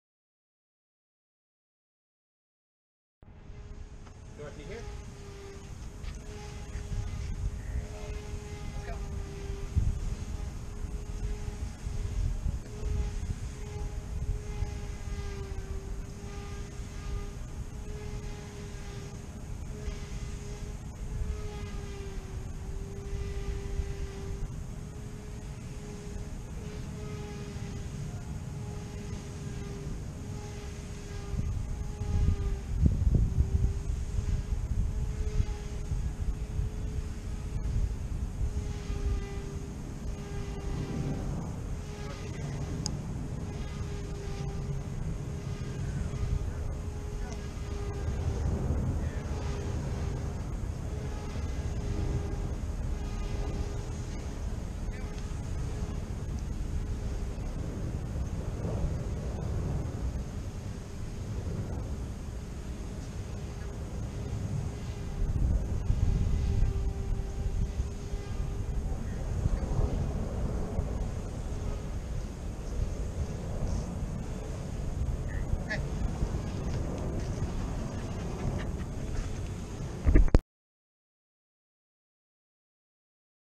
Dorothy, Border Collie puppy, during a dog training class in Memphis with Dog Obedience College
Young Border Collie puppy Dorothy practicing some of the skills she learned while attending the Advanced Off Leash Obedience course with Dog Obedience College in Memphis, TN